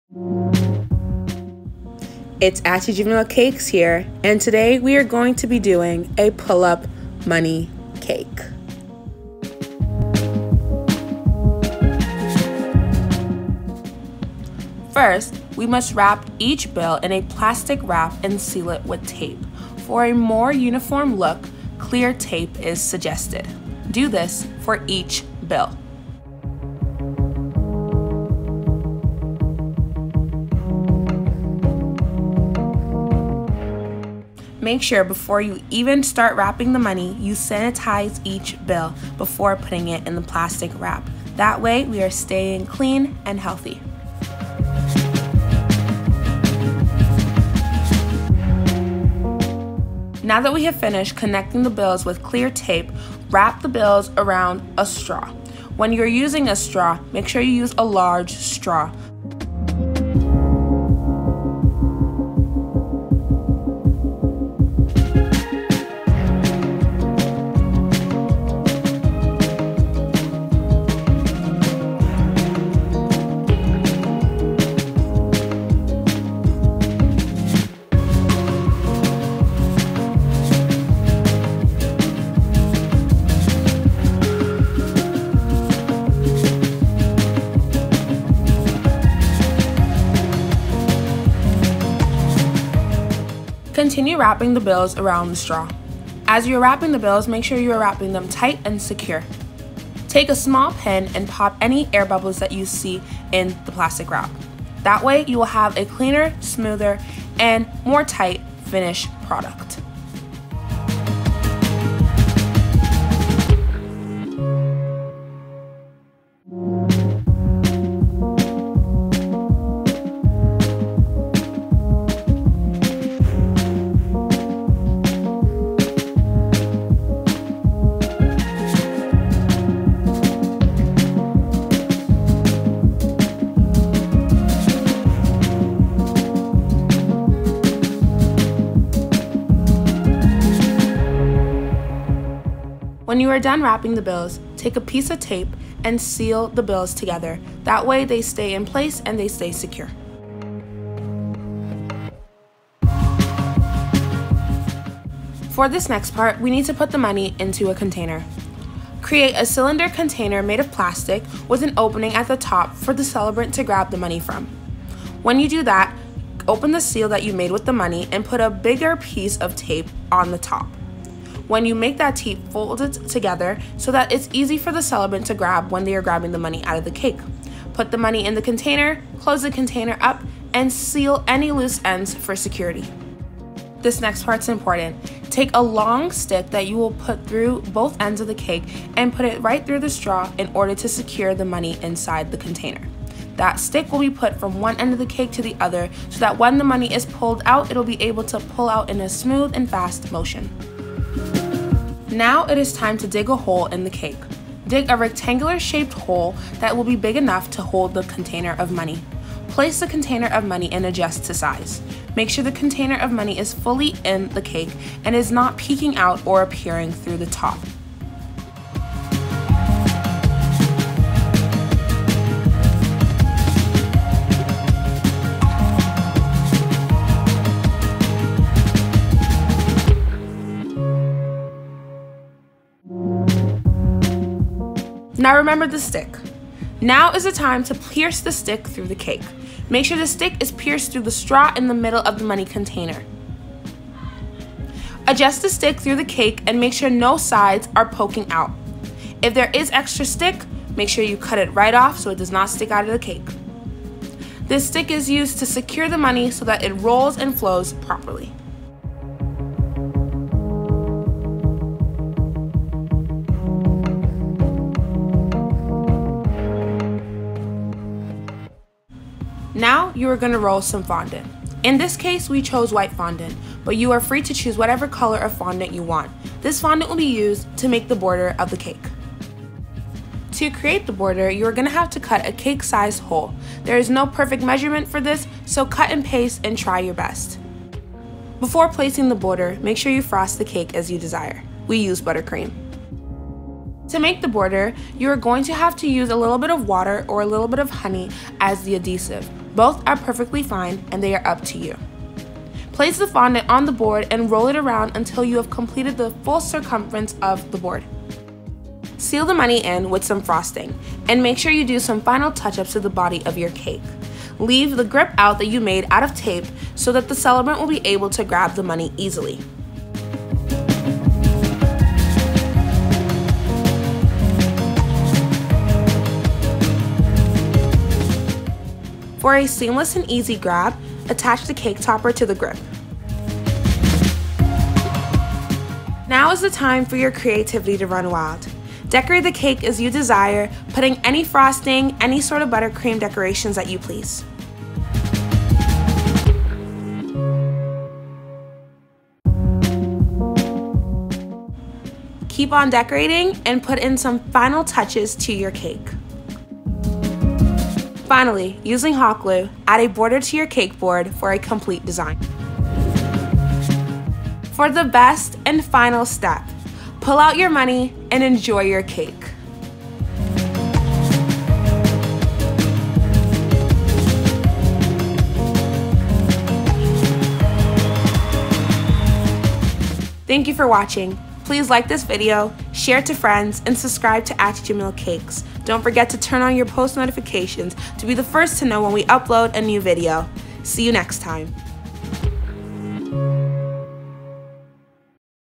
Pull-Up Money Cake TUTORIAL | Aty-Jimil Cakes
Learn how to make your own pull-up money cake, complete with bills, bills, bills.